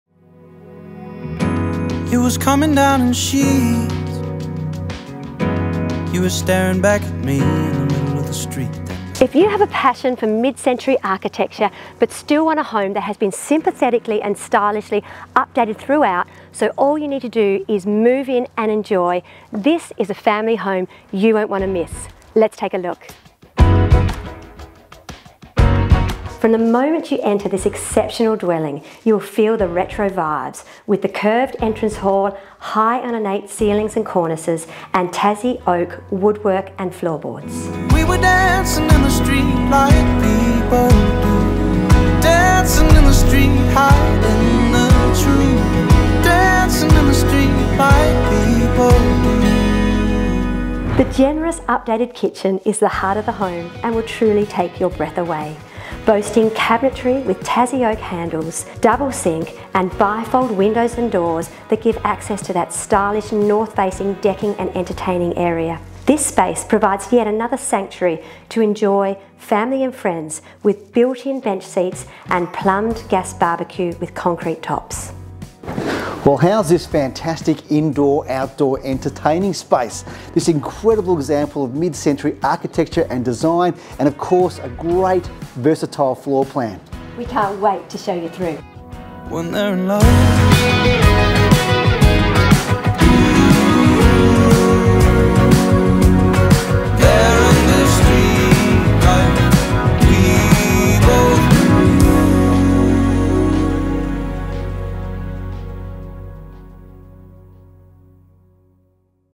36 Emily Avenue, Clapham | Presented by Simon & Bec Tanner
If you have a passion for mid-century architecture but want a home that has been sympathetically and stylishly updated throughout so all you need to do is move in and enjoy; then this family home is one you won’t want to miss.

Simon Tanner
Principal | Tanner Real Estate